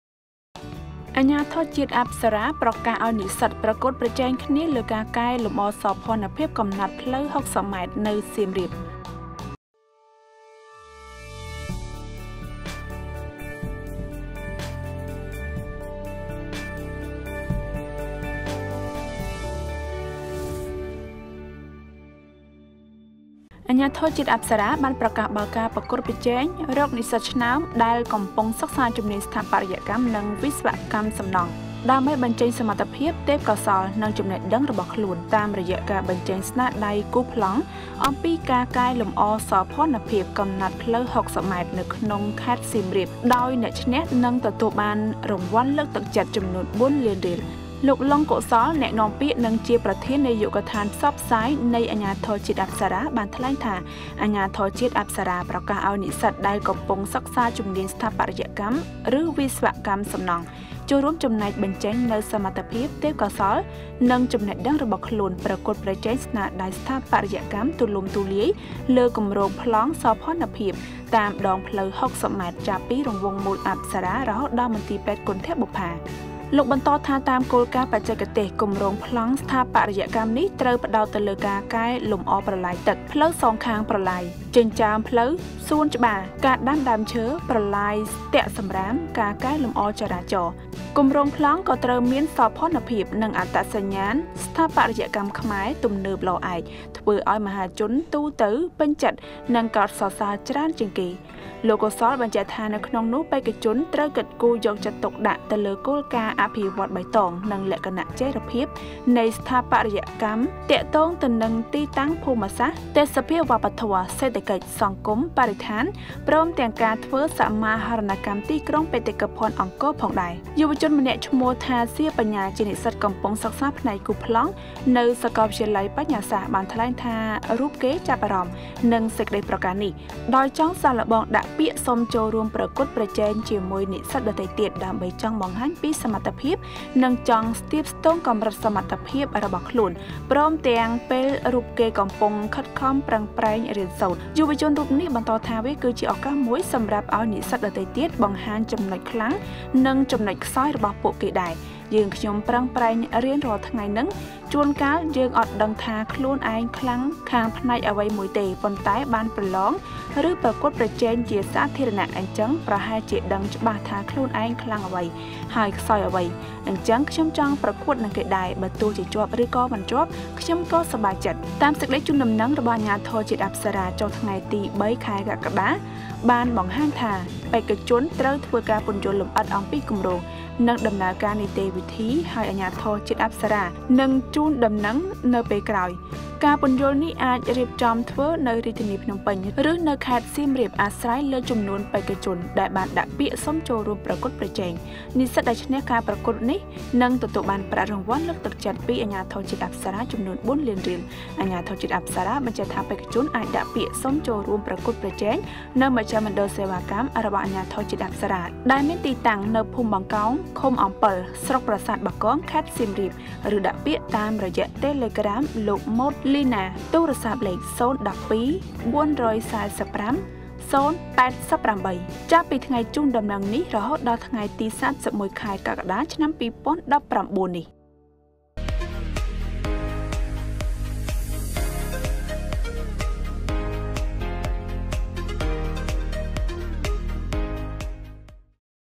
អាជ្ញាធរជាតិអប្សរា៖ ប្រកាសឲ្យនិស្សិតប្រកួតប្រជែងគូសប្លង់កែលម្អសោភ័ណភាពកំណាត់ផ្លូវ៦០ម៉ែត្រនៅសៀមរាប
អាជ្ញាធរជាតិអប្សរា បានប្រកាសបើកការប្រកួតប្រជែងរកនិស្សិតឆ្នើមដែលកំពុងសិក្សាជំនាញ​ស្ថា​បត្យកម្ម ឬវិស្វកម្មសំណង់ ដើម្បីគូសប្លង់ (Design) អំពីការកែលម្អសោភ័ណភាពកំណាត់ផ្លូវ៦០ម៉ែត្រ នៅក្នុងខេត្តសៀមរាប...

► តាមដានព័ត៌មានថ្មីៗរបស់ KHB Media តាមរយៈបានសង្គមខាងក្រោម៖

- ទស្សនាវីដេអូផ្សេងៗទៀត៖